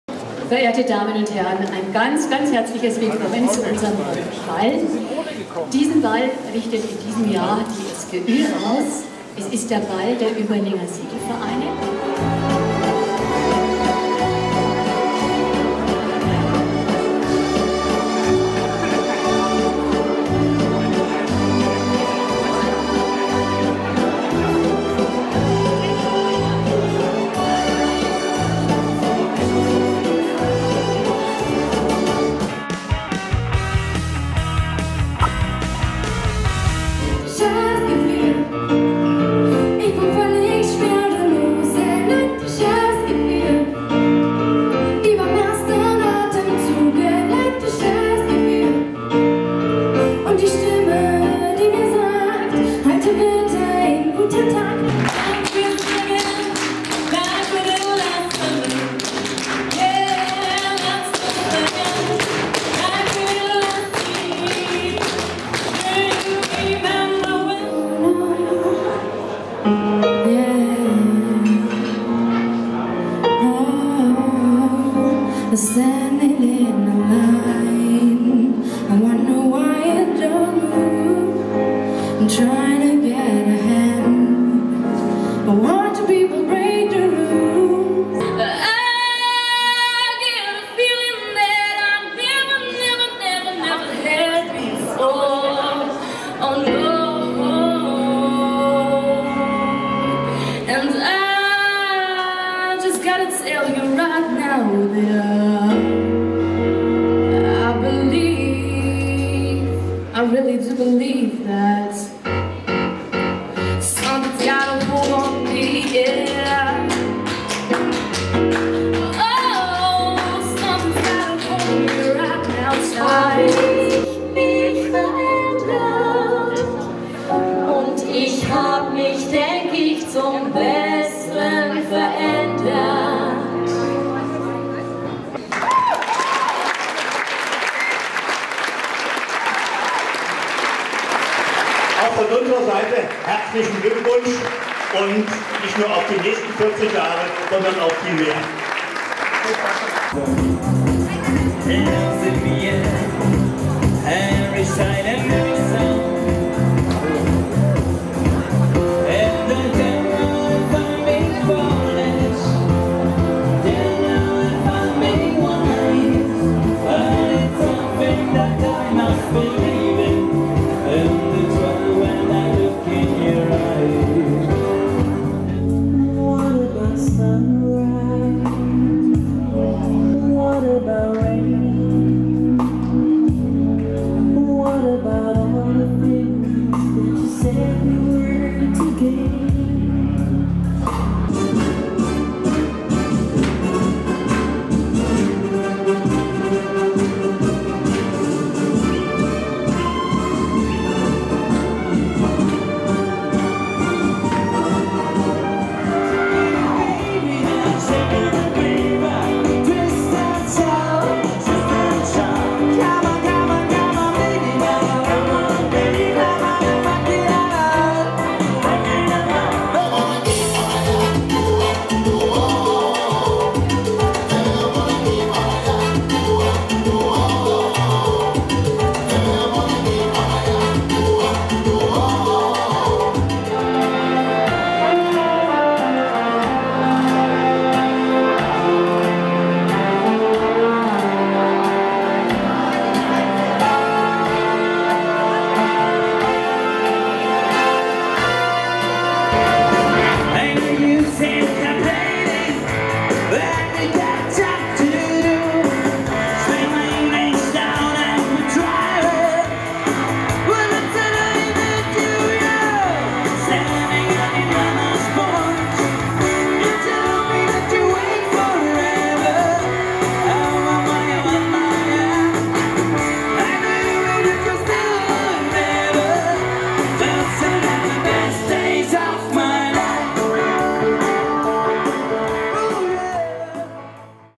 Seglerball 2012 in Überlingen
Am 17. November 2012 fand im Kurhaus in Überlingen der Seglerball statt. Neben vielen Möglichkeiten das Tanzbein zu schwingen, gab es auch einige Einlagen von Standart Tanz bis hin zu jungen Talenten der Musical Schule Überlingen.